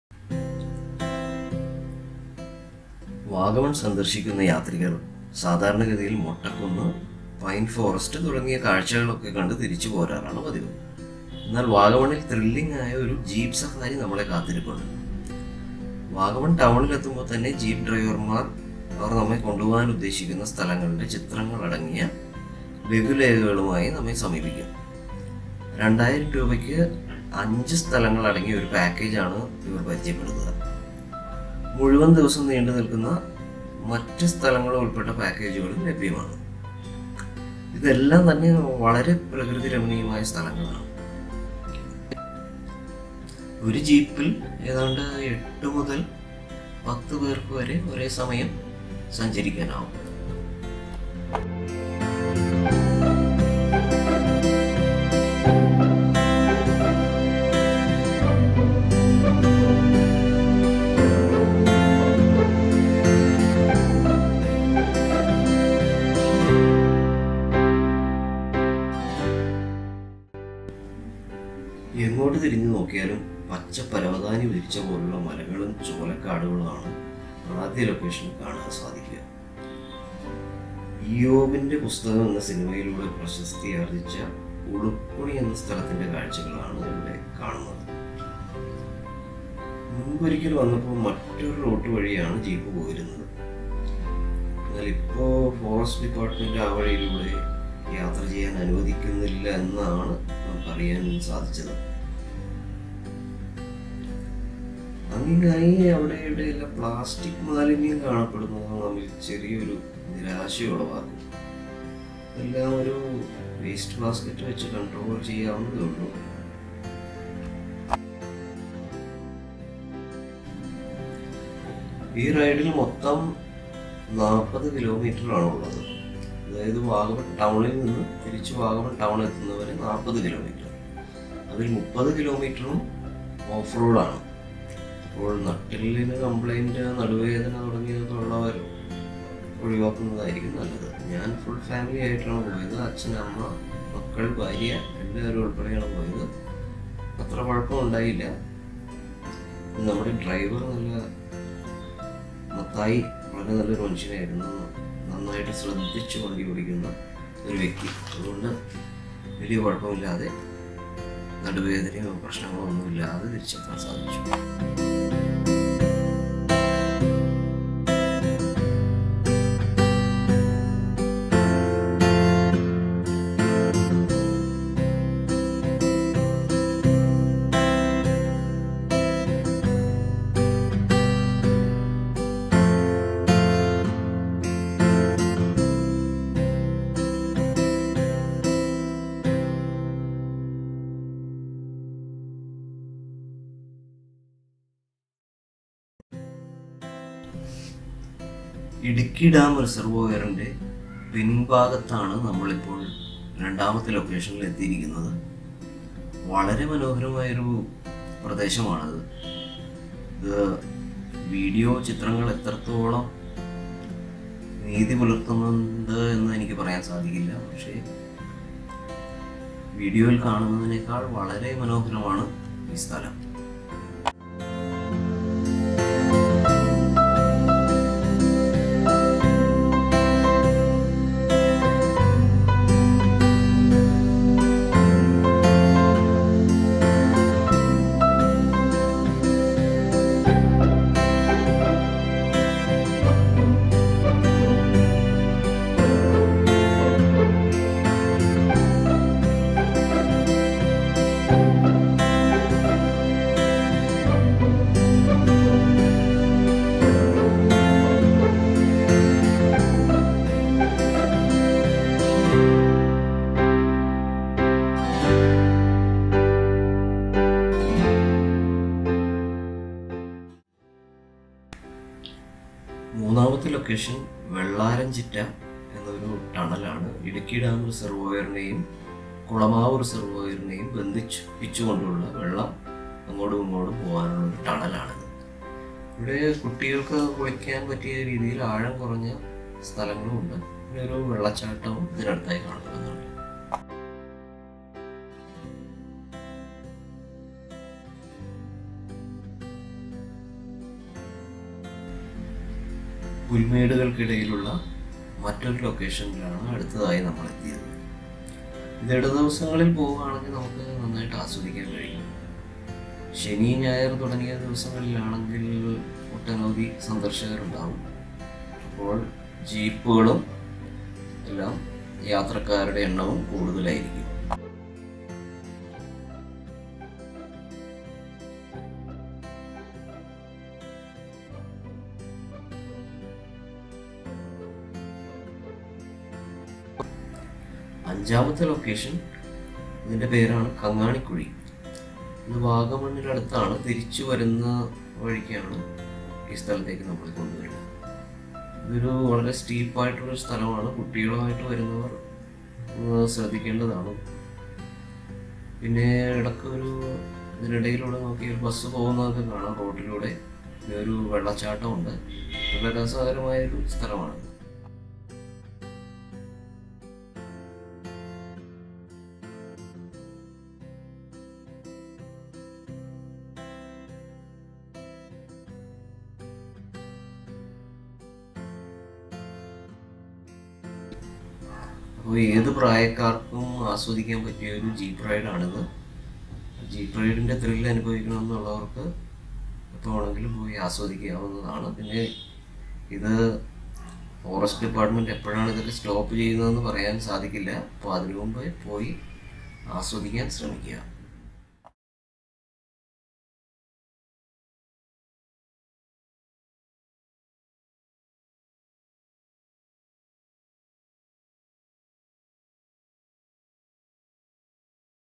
Vagamon jeep safari
Vagamon is a beautiful hill station located in Idukki district of Kerala,India.
One could opt for an offroad ride consisting of 40 kms in a hired vehicle.It costs aabout Rs 2000, covering 5 places.All the places included in this trip are very beautiful and breath taking..